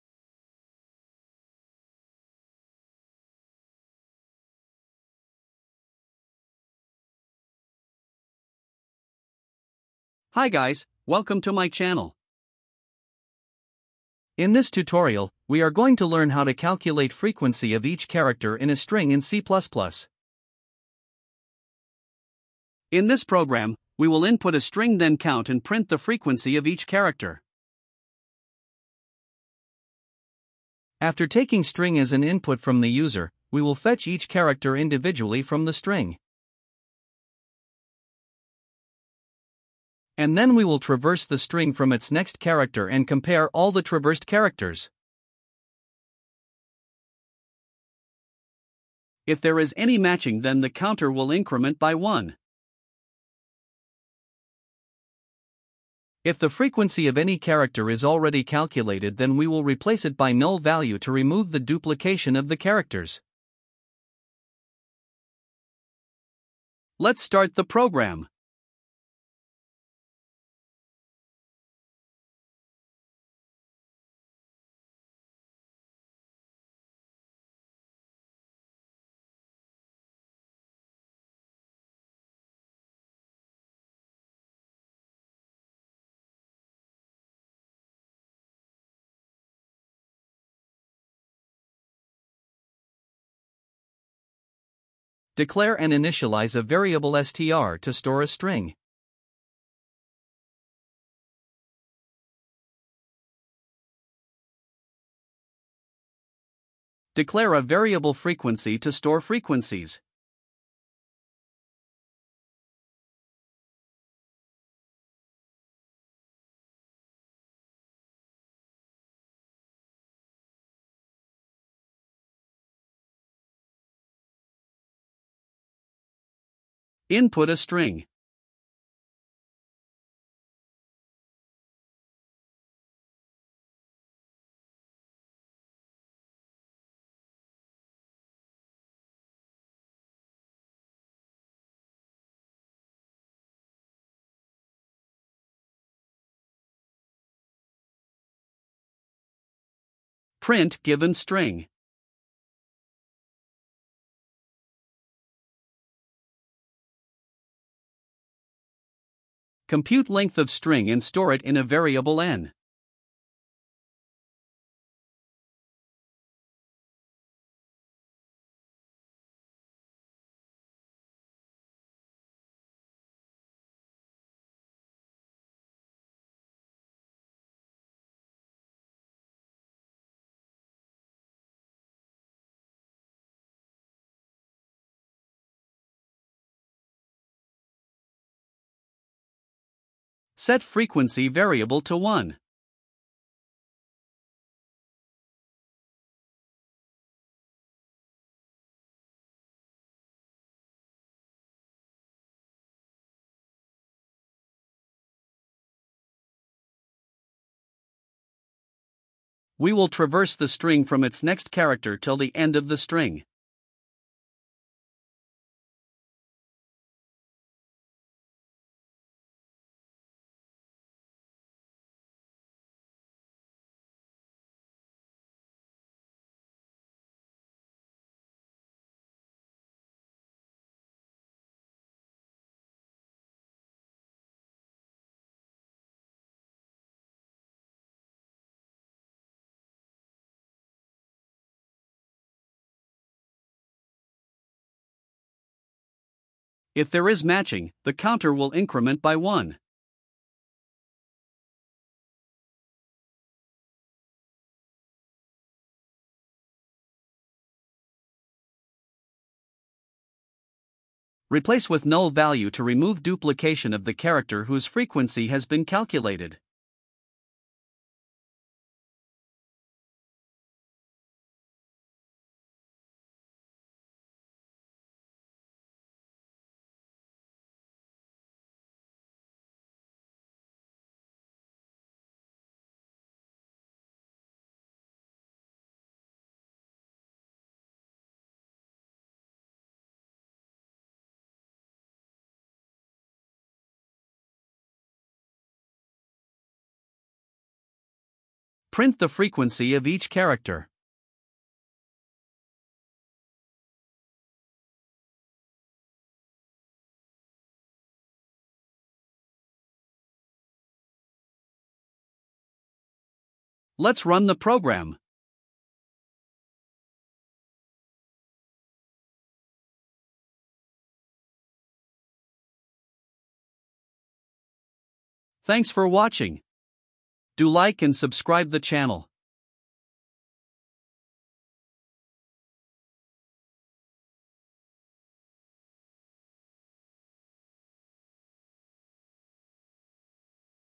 How to find frequency of each character in a string in C++
Hi everyone! Today we are going to learn how to find frequency of each character in a string in C++. This is simple and easy to understand tutorial. Keep Supporting. @programmingwithannu Thanks! :)